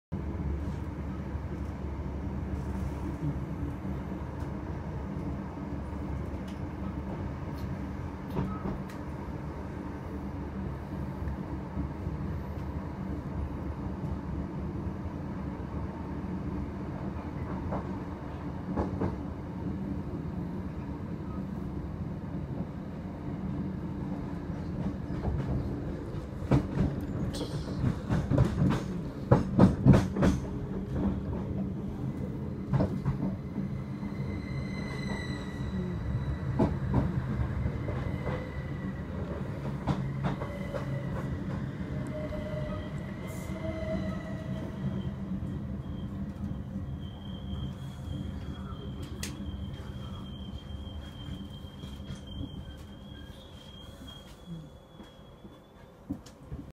Greater Anglia Class 317 Arriving Seven Sisters
Heading Southbound Service to London Liverpool Street